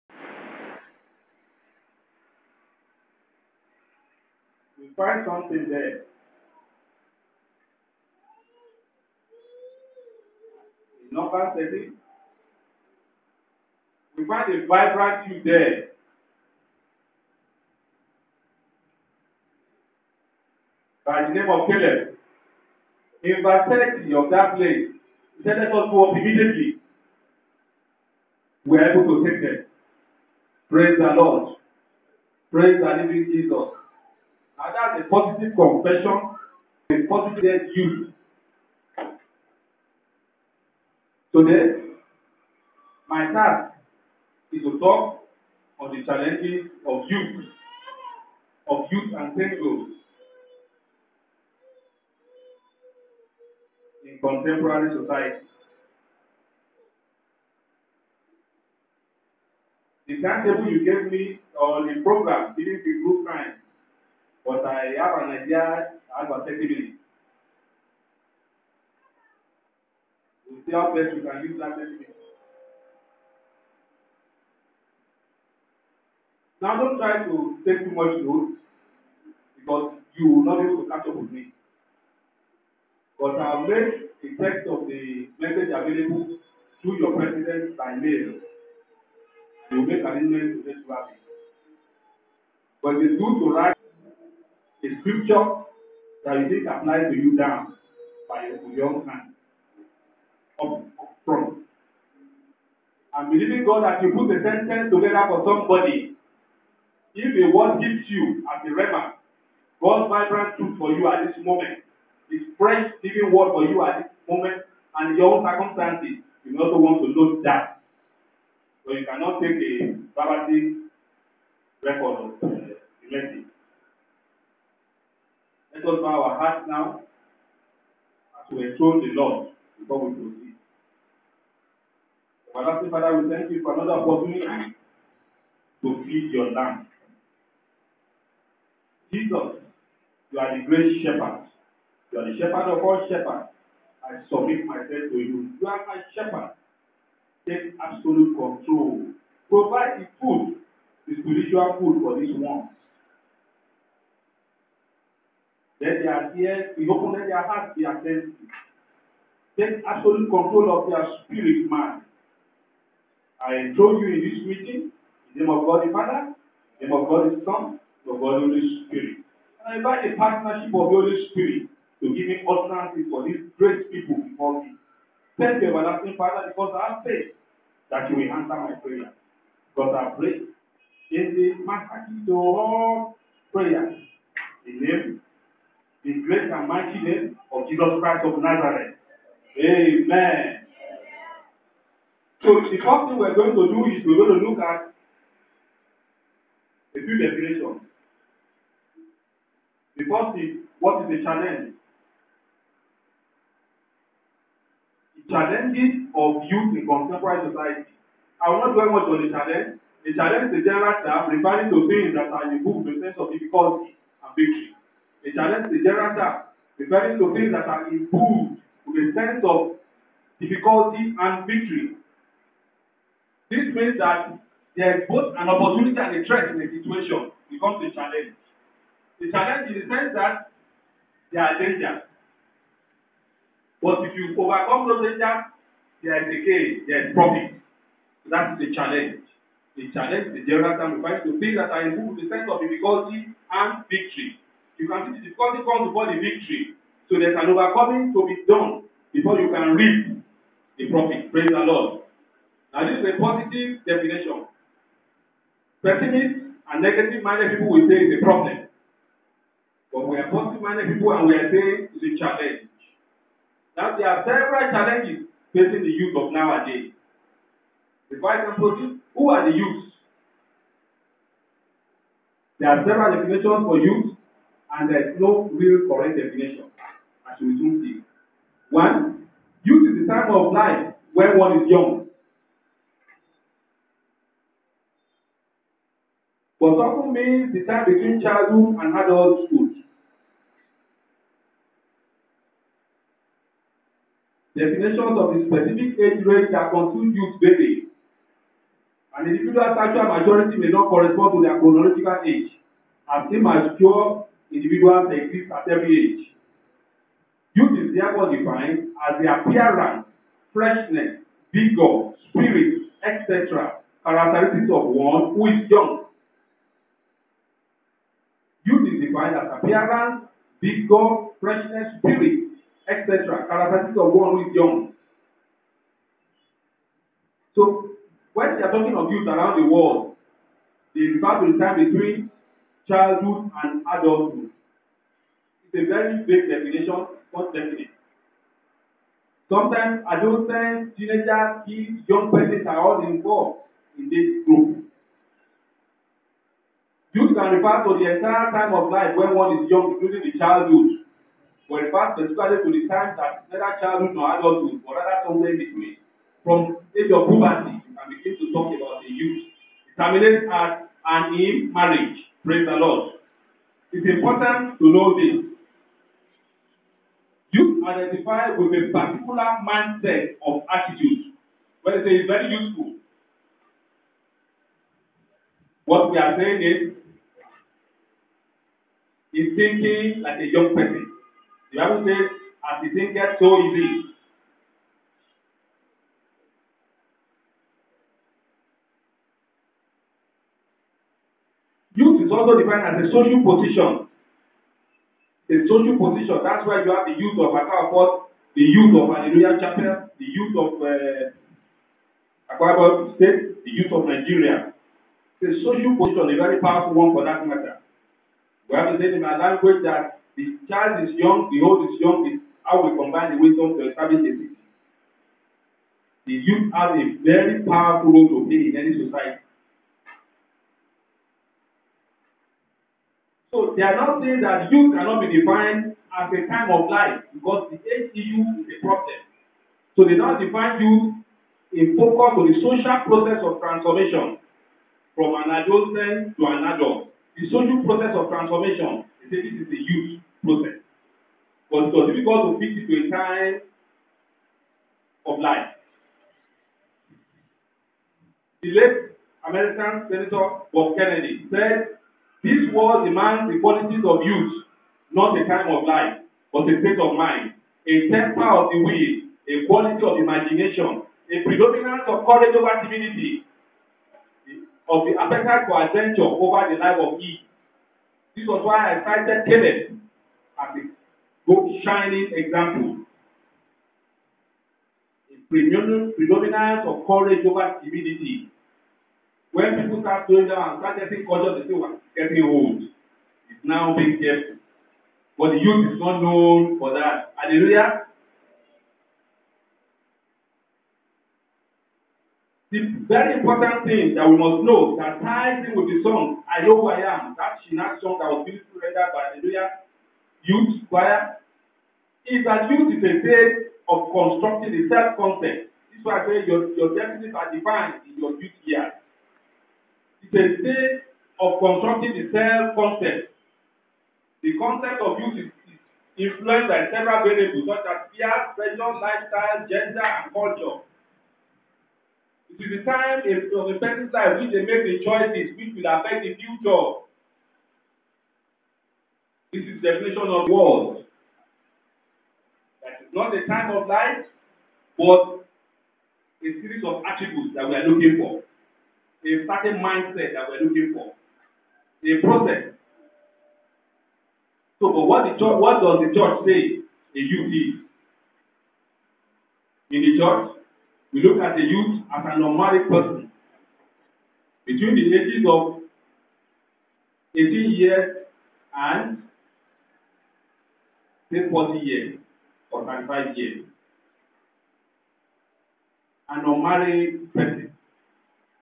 Youth,You have not been Devalued! - Deacon Taiwo
Deacon Taiwo Ogunkoya, APICP, RCCG HCU/ Founder K.O.K.NET, addressing the Youths of Halleluyah Chapel, Uyo recently.